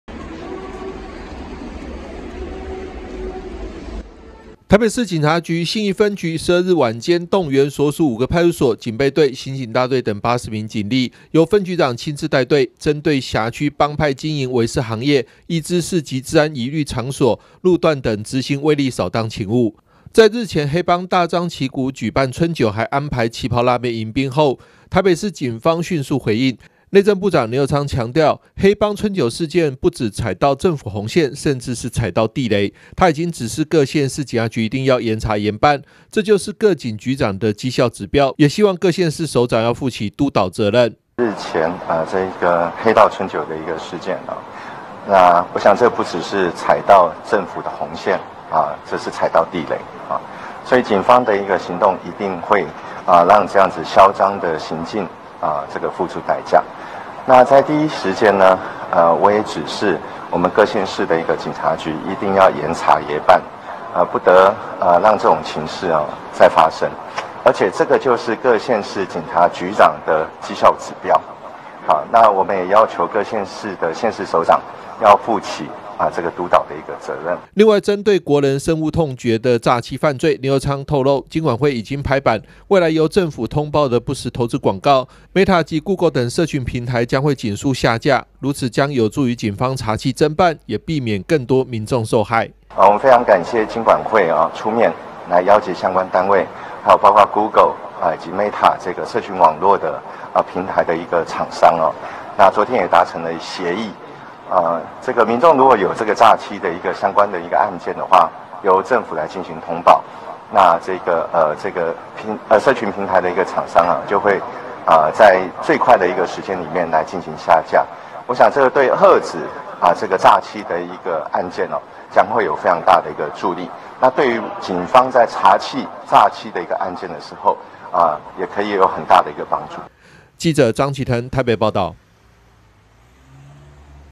黑道 林右昌|春酒 旗袍辣妹|臨檢 酒店|警力 掃蕩|詐欺 社群平台|2023 中嘉新聞|嗆黑道踩政府紅線 林右昌令各縣市警局嚴辦 囂張行徑必付代價
黑道春酒辣妹爭議 林右昌要求各縣市警局加強臨檢掃蕩 為囂張行徑付出代價

（中嘉新聞記者張啓騰/台北報導）日前有黑幫在台北市五星級酒店舉辦春酒，還安排辣妹迎賓，囂張氣焰引起社會關注，內政部長林右昌表示黑道春酒事件不只採到政府紅線甚至是踩到地雷，警方行動一定會讓這項囂張行徑付出代價，他已經指示各縣市警局一定嚴辦嚴查，這就是各警局長的績效指標，另外林右昌也透露金管會已經拍板定案由政府舉報的不實投資廣告，臉書等社群媒體將儘速下架，這對嚇阻詐欺案件將有相當的助力對於警方在查緝詐欺案件也會也很大的幫助。

台北市警局信義分局12日晚間動員所屬五個派出所、警備隊、刑警大隊等80名警力，由分局長親自帶隊，針對轄區幫派經營、圍事行業、易滋事及治安顧慮處所、路段等執行威力掃蕩勤務，在日前黑幫大張旗鼓舉辦春酒還安排旗袍辣妹迎賓後，台北市警方迅速回應，連續數日舉辦大規模臨檢掃蕩，內政部長林右昌強調這不只是採到政府紅線甚至是踩到地雷，他已經指示各縣市警察局一定要嚴查嚴辦，這就是各警局長的績效指標，也希望各縣市長要負起督導責任。

另外針對國人深惡痛絕的詐欺犯罪，林右昌透露金管會已經拍板，未來由政府通報的不實投資廣告，meta及google等社群平台將會儘速下架，如此將有助於警方查緝偵辦也避免更多民眾受害。